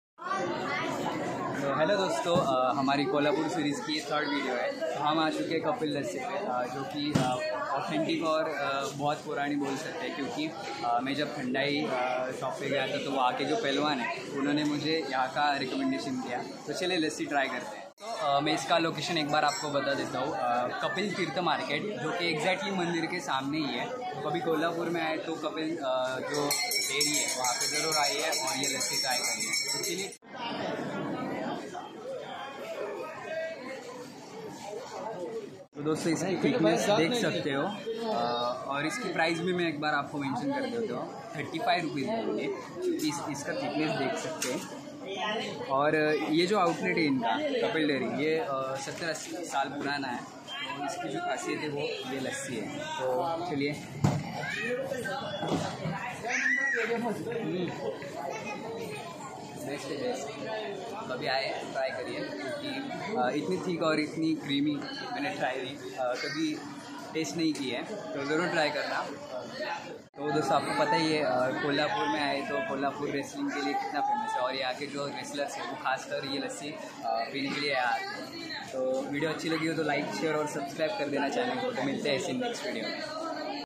लस्सी | Best lassi in kolhapur | कितनी थिक लस्सी है यार😋 | Punjabi lassi recipe | Food journey
hello friends welcome to food journey to kholhapure mai agara aye or yeh kapil lassi try nhi kiya tho jarur krna Kita thick or creamy hai yeh I hope you like the video thanks for watching 🙏😁😍

location 📍
Kapil Tirth Market
3078, Tarabai Rd, Shivaji Peth A Ward, C Ward, Kolhapur, Maharashtra 416012